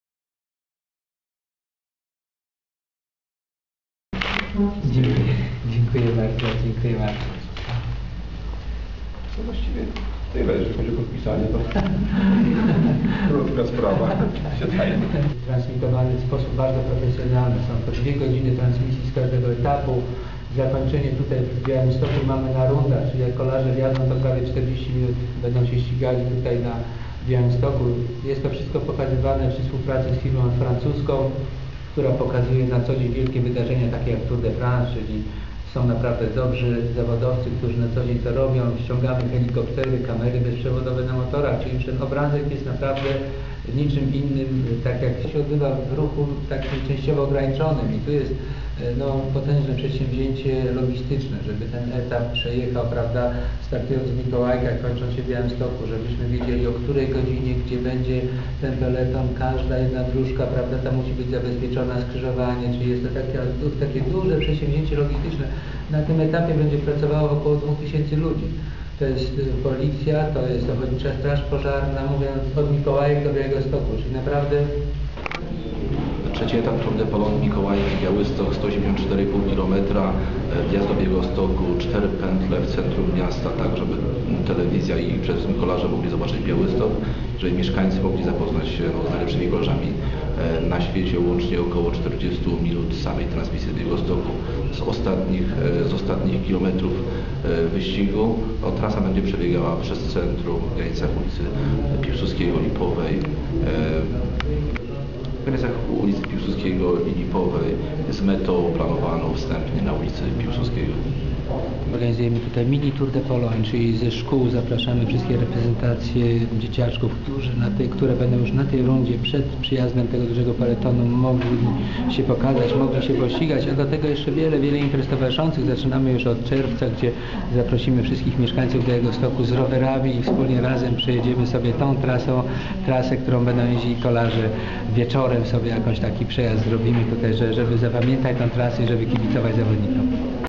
Tour de Pologne w Białymstoku
Trasa wyścigu kolarskiego Tour de Pologne przebiegać będzie przez Białystok